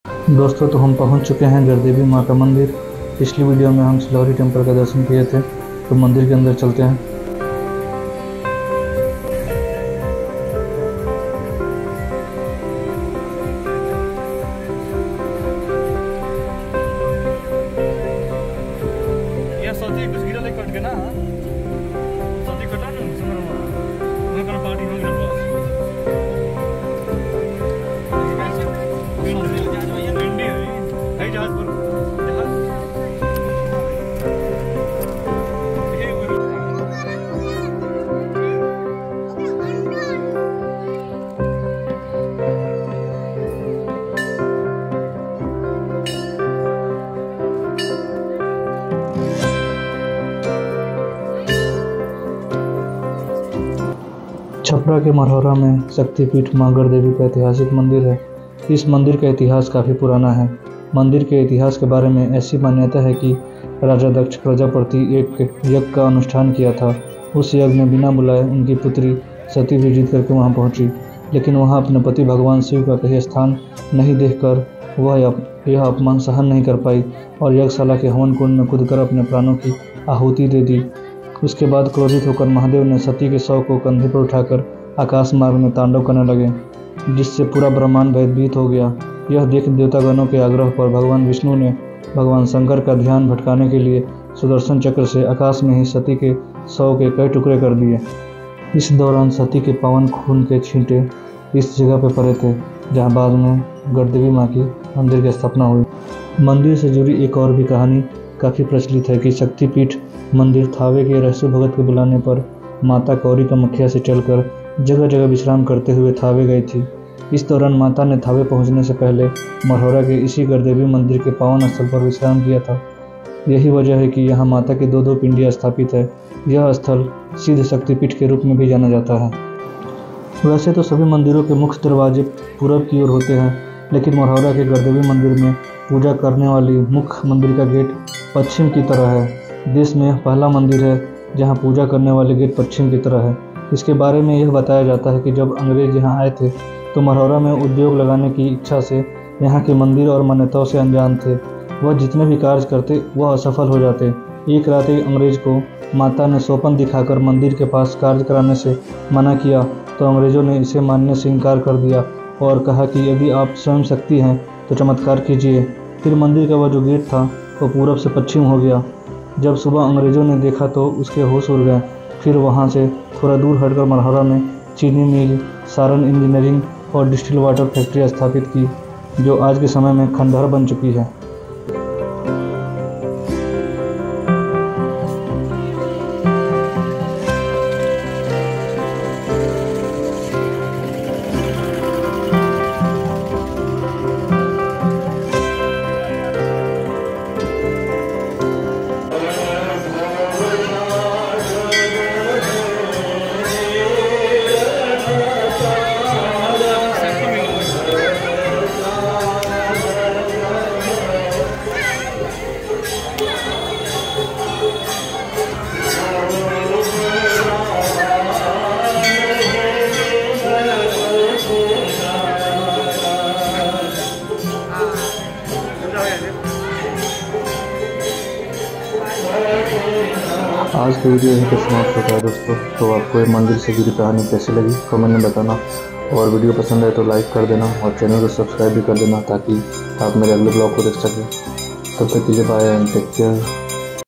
गढ़देवी मंदिर का रहस्य | The mystery of Garh Devi Mandir
इस वीडियो में, हम आपको गढ़देवी मंदिर के इतिहास और महत्व के बारे में बताएंगे, जो बिहार की धारों में छुपी रहस्यमयी कहानियों की एक झलक प्रस्तुत करता है।